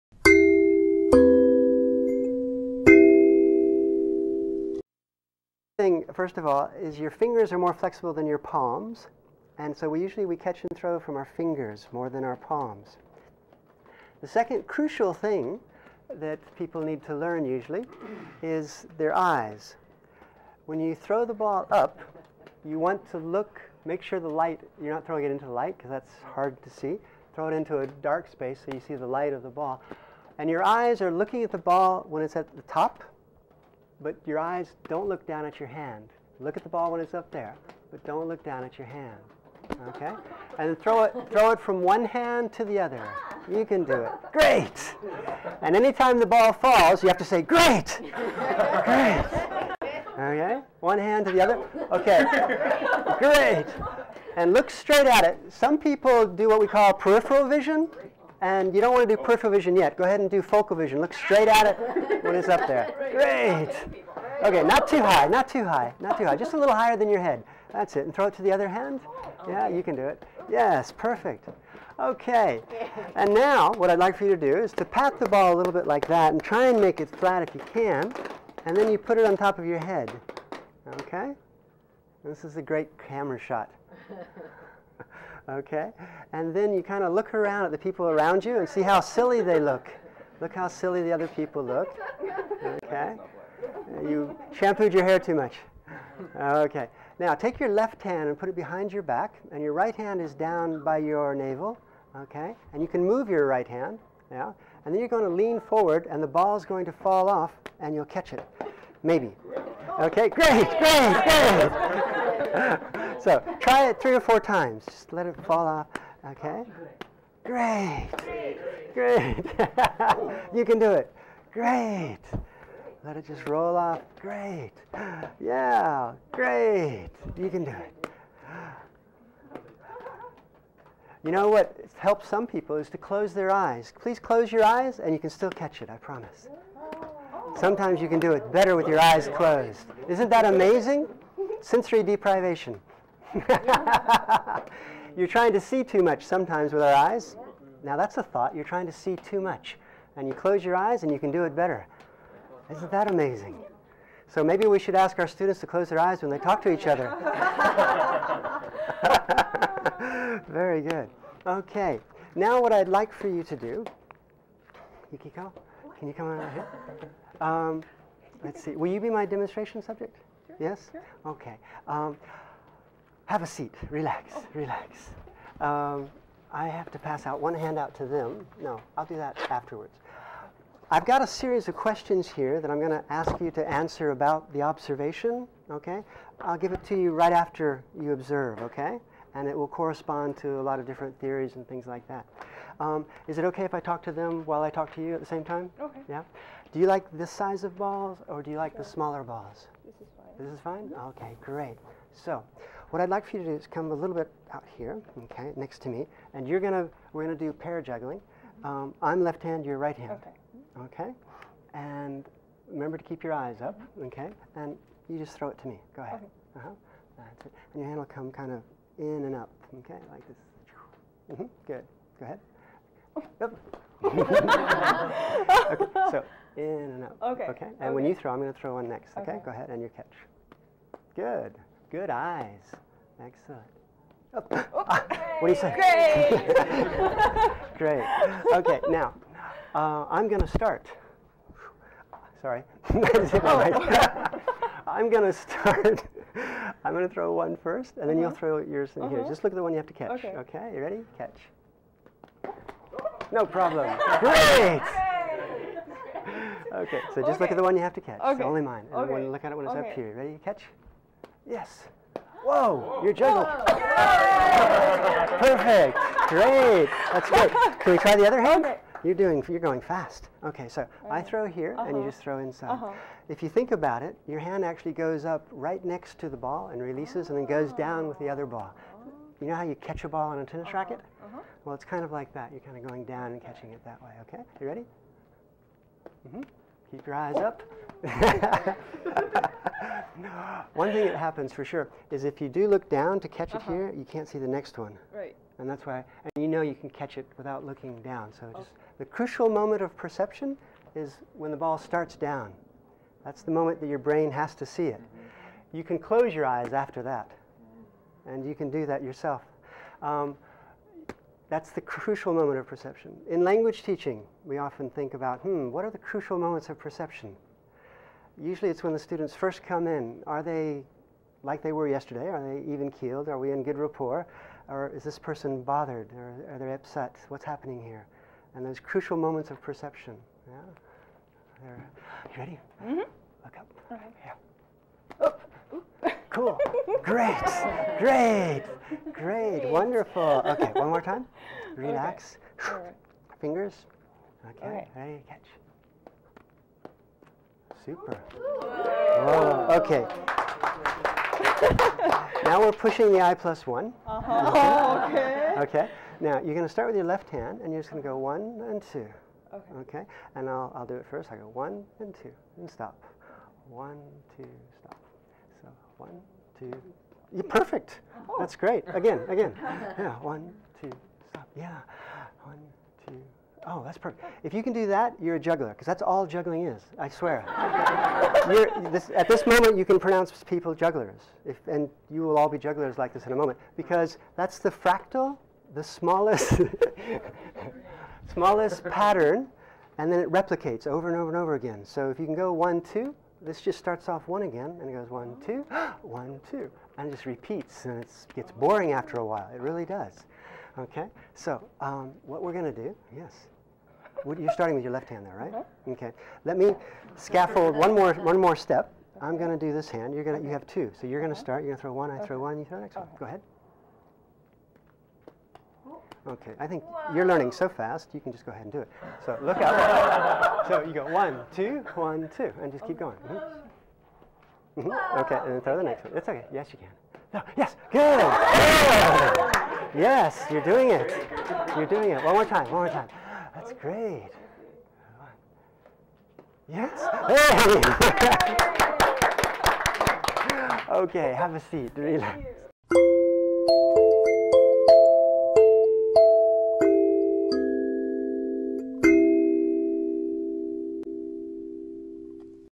How to Juggle: Juggling with Language Learning Theories
To download the full video free go Dr. Tim Murphey shows how effective steps for learning to juggle often parallel steps for learning foreign languages, or, indeed, learning anything. Academic theories of language learning (Krashen, Vygotsky, and others) can be made more concrete with experiential information. When language learners learn to juggle, it can facilitate beliefs and attitudes that support learning and help them become more meta-cognitive.  For more information about this license, please read